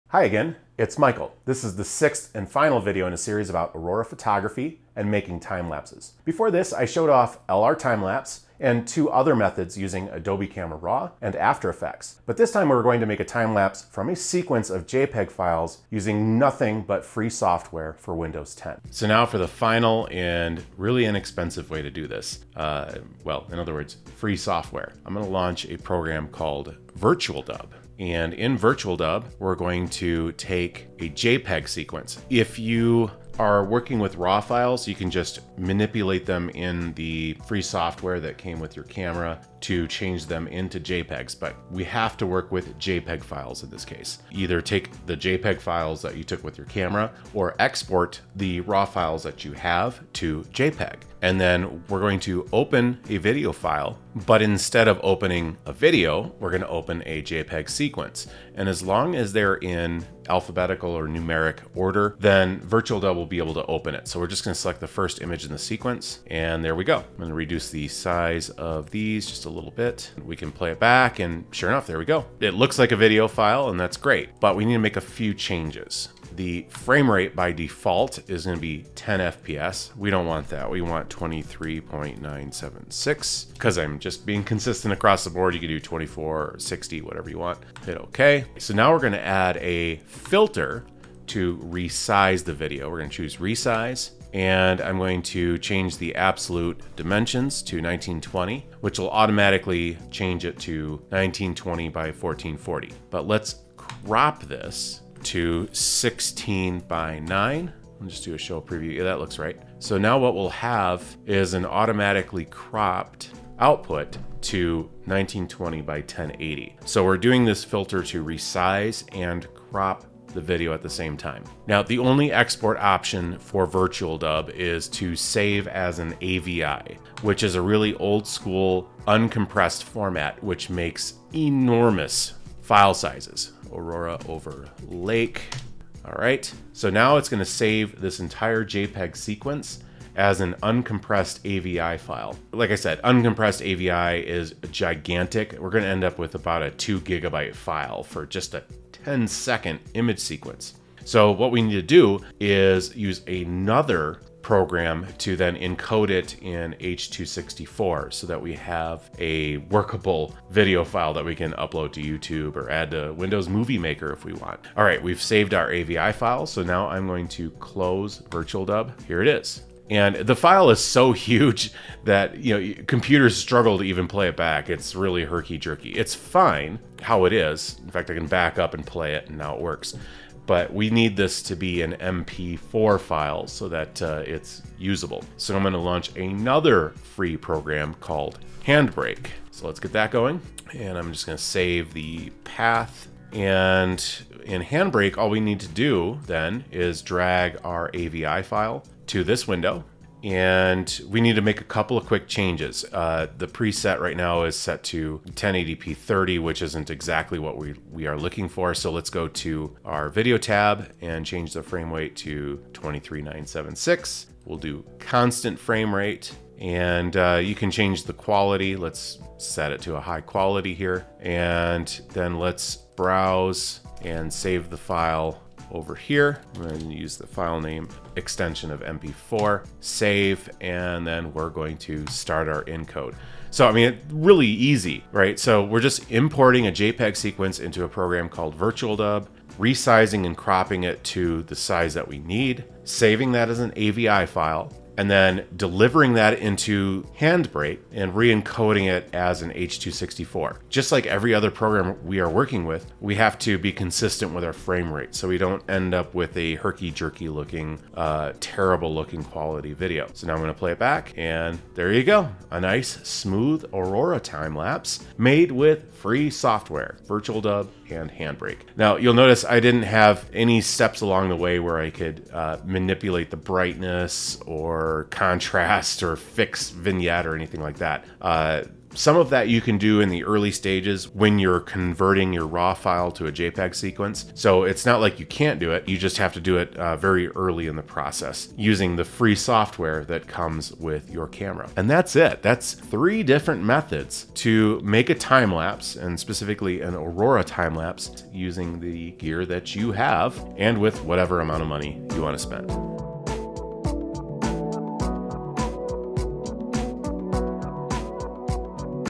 Making Timelapses 4: The "Free" Method. VirtualDub and Handbrake
In this video I create an Aurora / Northern Lights timelapse using nothing but "Free Software" on a Windows 10 PC.  VirtualDub for assembling the image sequence into AVI and Handbrake for converting the AVI file into a usable MP4 file.